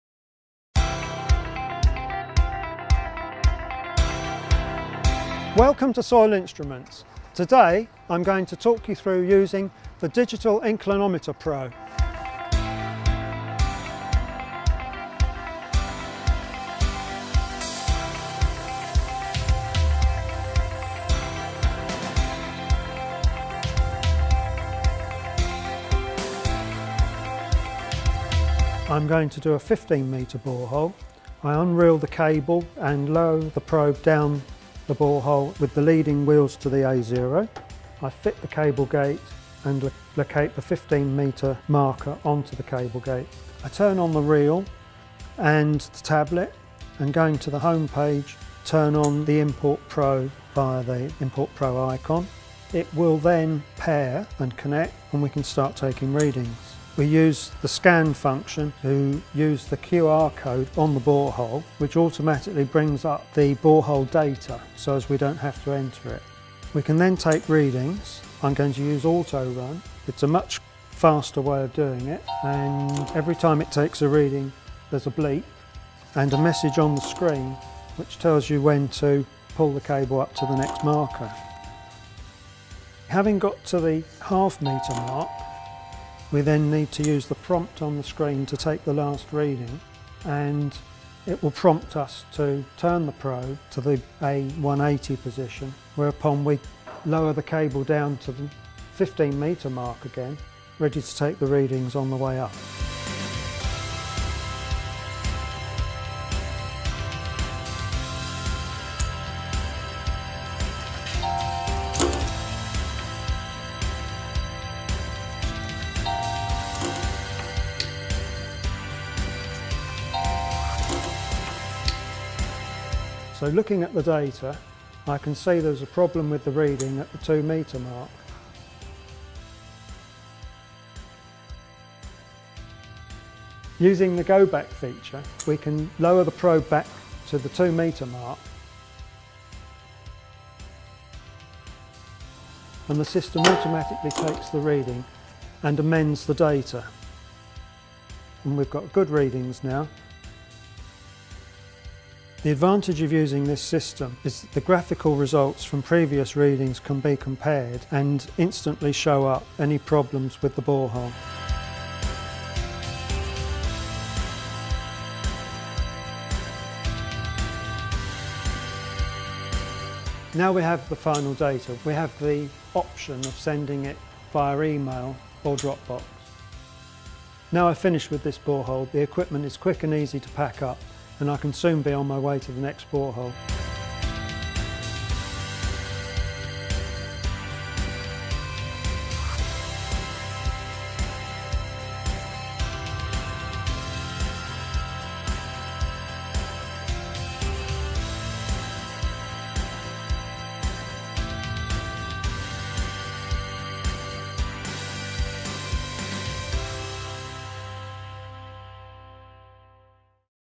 soil instruments digital inclinometer pro
Soil Instruments' new C17 PRO digital inclinometer, now available from Geomotion Australia.

The Vertical Digital Inclinometer Pro System is used to measure lateral deflections within a borehole. The system comprises a biaxial probe, cable reel and ultra-rugged Field Tablet supplied with ‘In-Port Pro’ data capture software.

The probe incorporates MEMS technology allowing highly accurate and repeatable readings, transferred via a digital signal. Bluetooth communication enables a cable free data transmitting system with no connectors to corrode or break.

The robust reinforced Kevlar® cable consists of a non slip cable marker system which, when used in conjunction with the cable gate, provides highly accurate and repeatable depth control.

With all these combined features, the Vertical Digital Inclinometer Pro System is a robust and highly accurate system that is light, compact and easy to operate in any site environment.